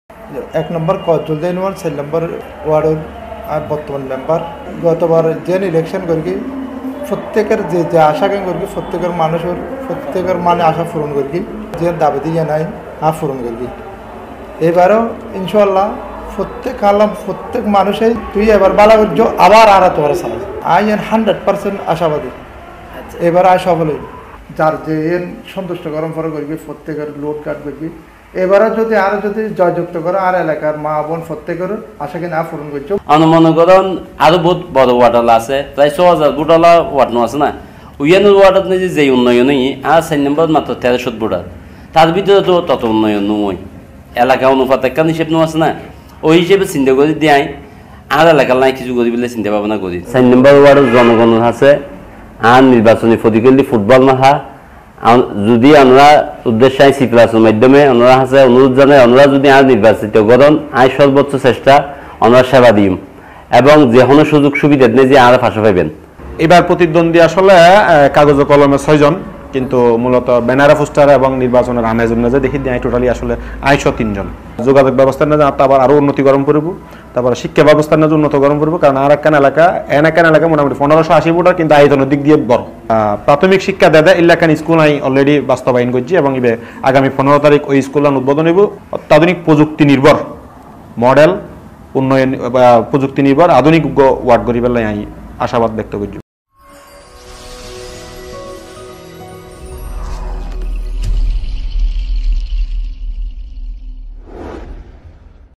কর্ণফুলীর জুলদা ইউনিয়নের ৪ ও ৯ নং ওয়ার্ড ইউপি সদস্য (মেম্বার) পদপ্রার্থীরা যা বললেন | Cplus
Slogan: আরাঁ চাটগাঁইয়া হতা হই
Country: Bangladesh
Language: Bengali।Chittagong Language
Broadcast area: Online